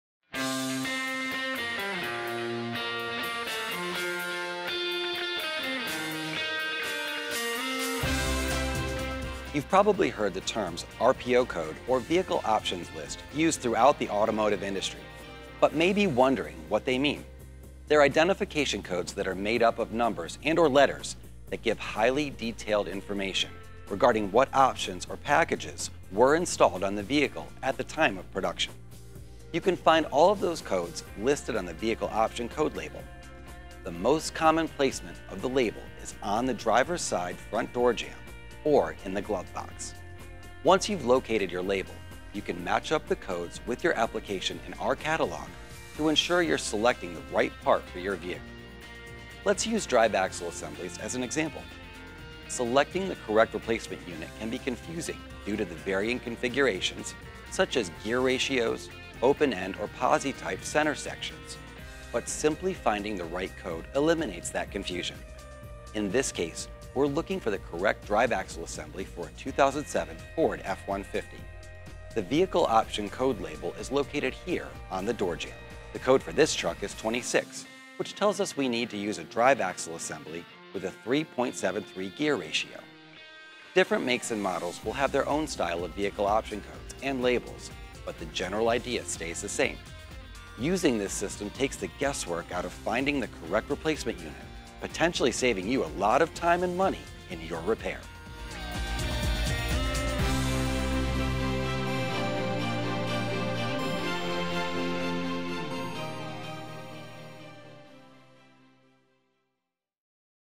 Drive Axle Assembly Vehicle Option Codes - Ensure You Have the Right Part for Your Vehicle
Vehicle option lists (or RPO codes) give highly detailed information regarding what options or packages were installed on the vehicle at the time of production.

This video shows how to locate and use vehicle option code labels to identify the correct replacement drive axle assembly (differential), as well as other parts on the vehicle.